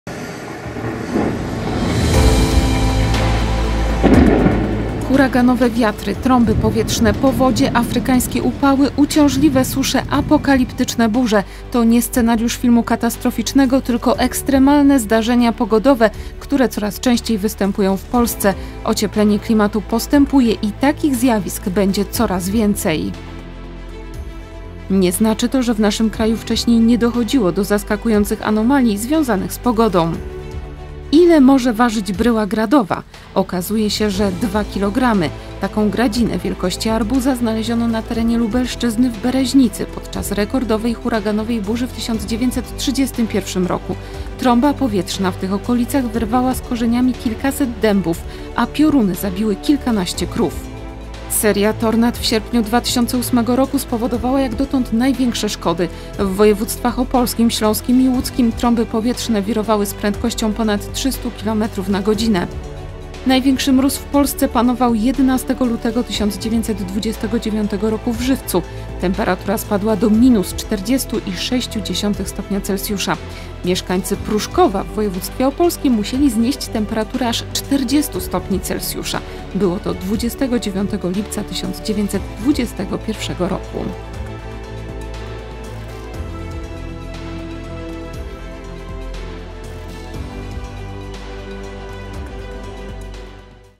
Zabójcza pogoda już w Polsce | 60 sekund
Huraganowe wiatry, trąby powietrzne, powodzie, afrykańskie upały, uciążliwe susze, apokaliptyczne burze – to nie scenariusz filmu katastroficznego, tylko ekstremalne zdarzenia pogodowe, które coraz częściej występują w Polsce. Ocieplenie klimatu postępuje i takich zjawisk będzie coraz więcej.